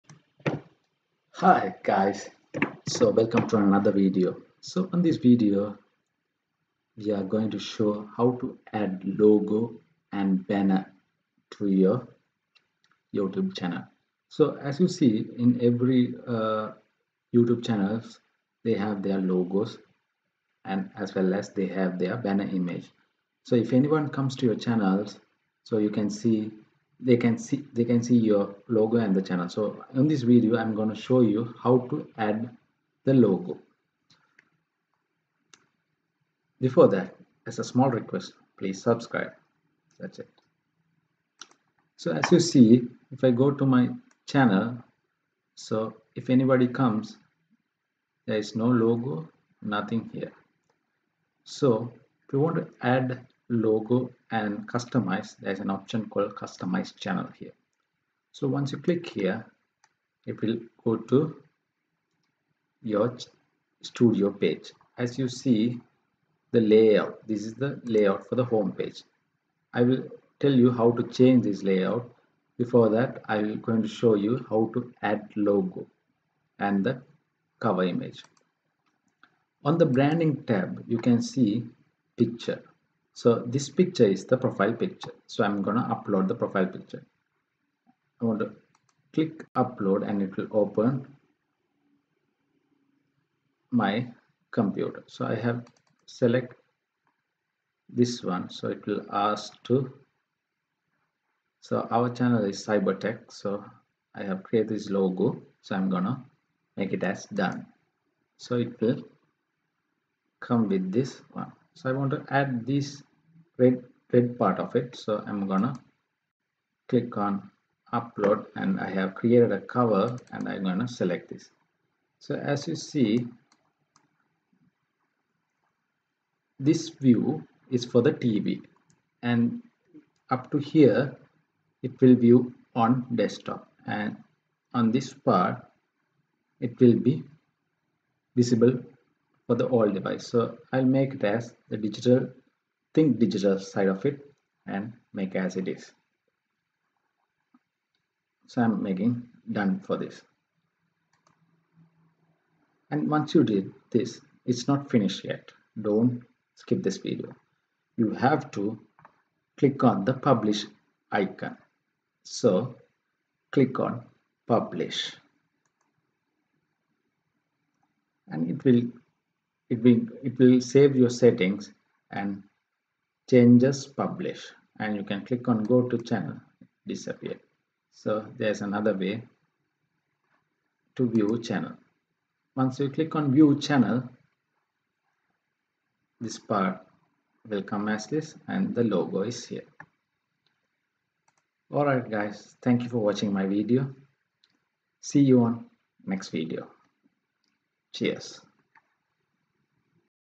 How to add logo and banner in Youtube 2021 | Manage your channel branding
Manage your channel branding
Change your profile picture in YouTube Studio. Your profile picture is the image shown to viewers on your channel, videos, and publicly attributable actions across YouTube.

From the left menu, select Customization and then Branding.
Click CHANGE and select an image. Change the size of your image, then click DONE.
Click PUBLISH. It may take a few minutes for your profile picture to update across YouTube.